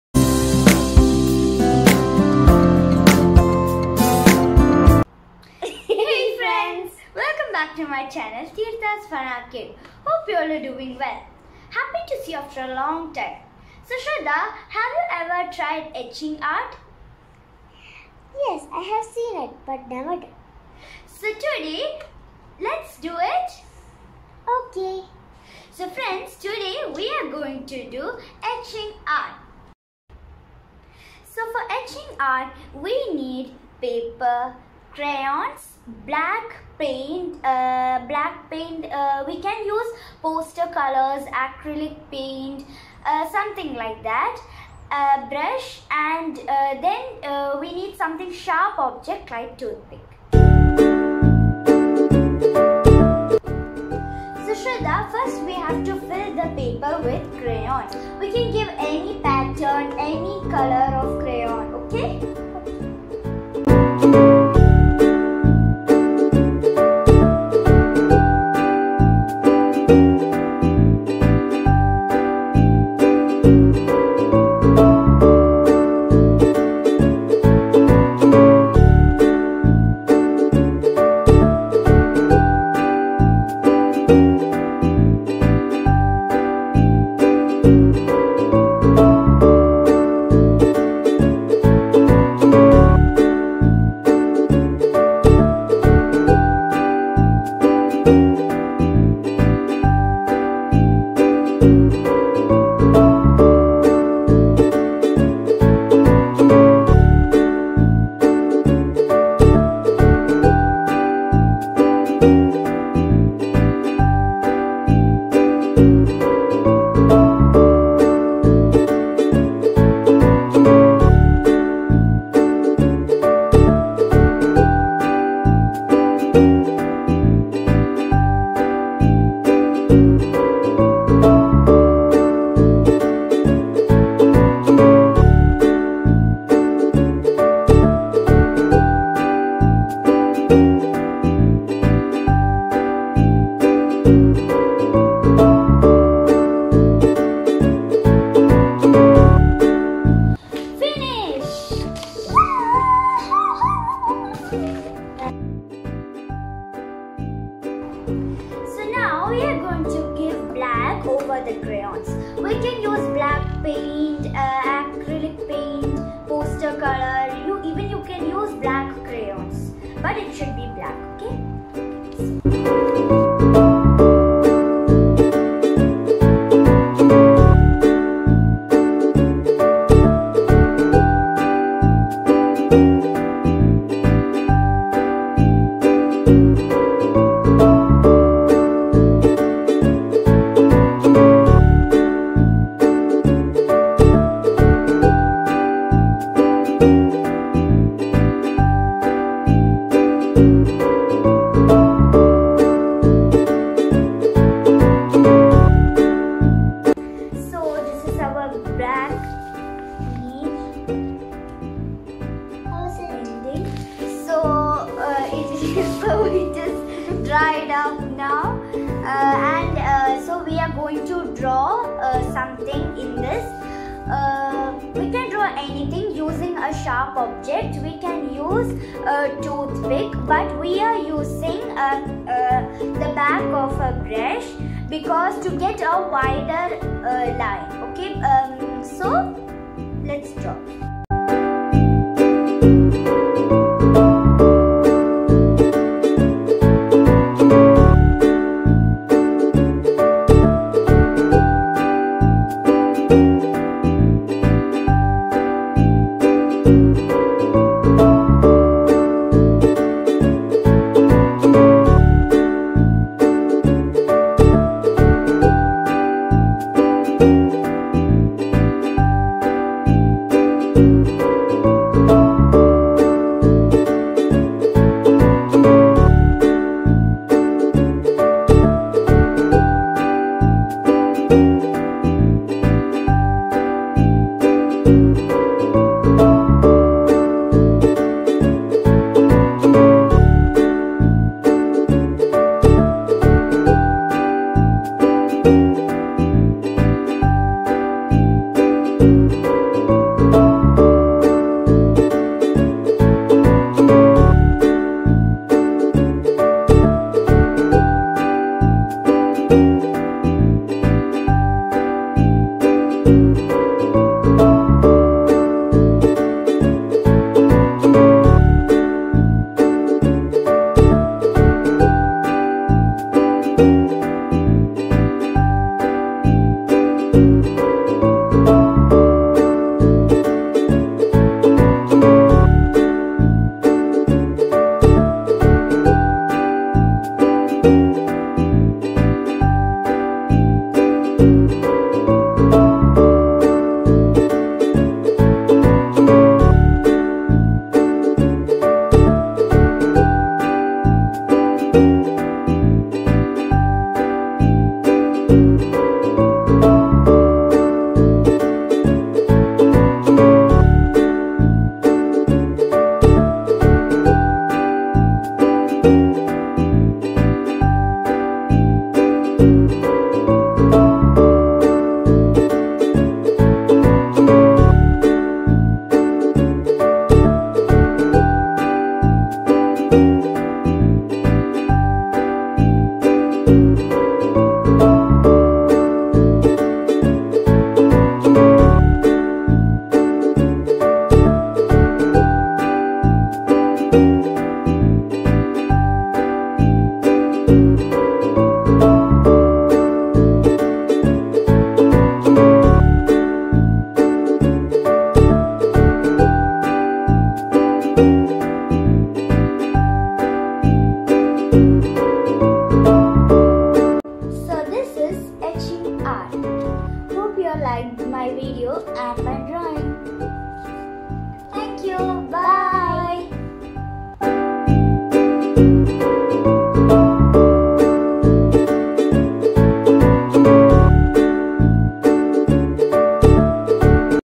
Etching art with crayons
Teerdha's Fun Arcade

Etching art technique is an interesting and fun art for kids. Make your own designs by etching technique. Materials required :

Paper
Crayons/ oil pastels
Black acrylic paint / poster color / black crayon / black oil pastel.
Any sharp thing like a tooth pick, scissors, used up pen...